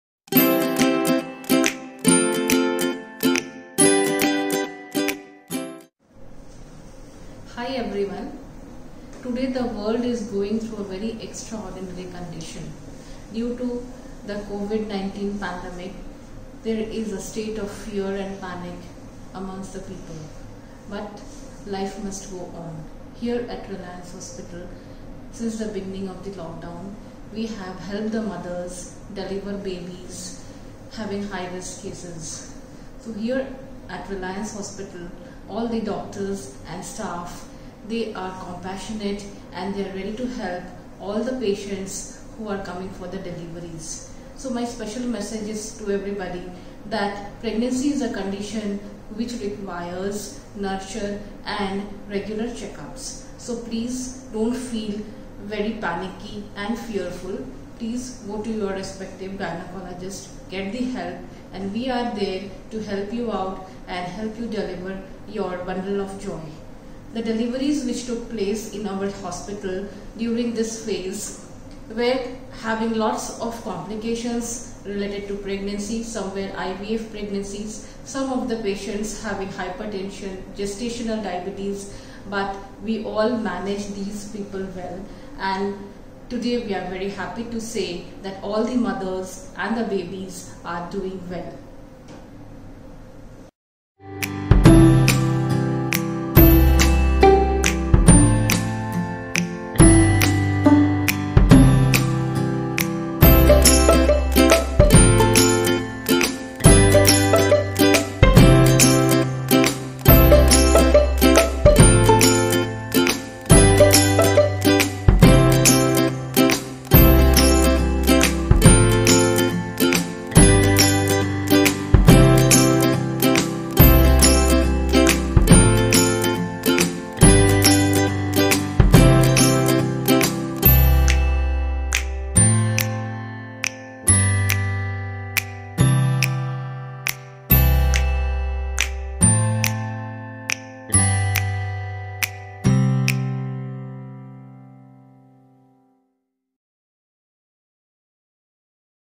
Lockdown Babies
Dr. Bandita Sinha, consultant, Gynecology and Obstetrics at Reliance Hospitals, Navi Mumbai shares the importance of safe and timely pregnancy care in the midst of the Covid-19 pandemic. Experts at our Department of Gynecology and Obstetrics have safely delivered many babies in this lockdown using compassionate care and advanced technology.